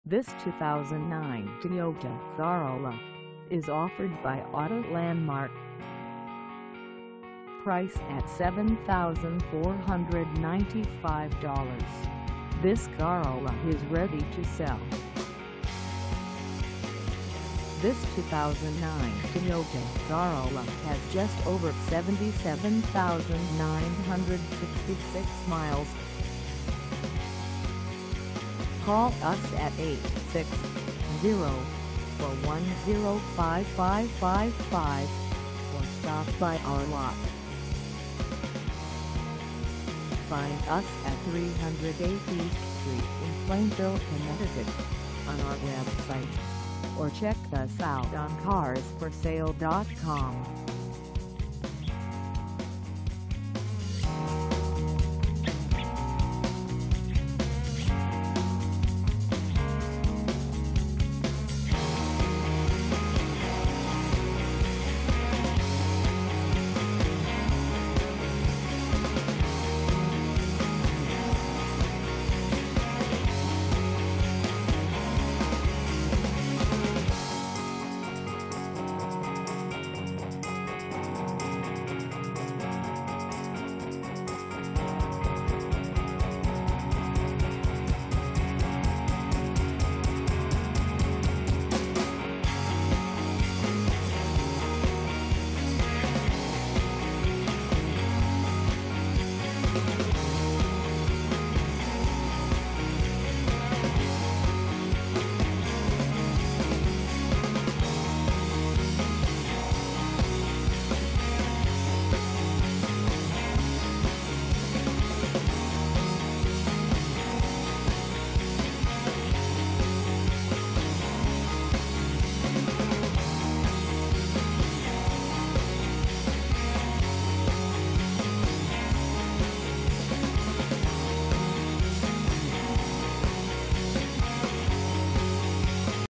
2009 Toyota Corolla LE 4dr Sedan 4A for sale in Plainville,
This 2009 Toyota Corolla LE 4dr Sedan 4A is for sale in Plainville, CT 06062 at Auto Landmark.

Engine: 1.8L I4
Transmission: Automatic 4-Speed
Color: Black
Mileage: 77966
Contact Auto Landmark 308 East Street Plainville, CT 06062 to test drive this 2009 Toyota Corolla LE 4dr Sedan 4A today.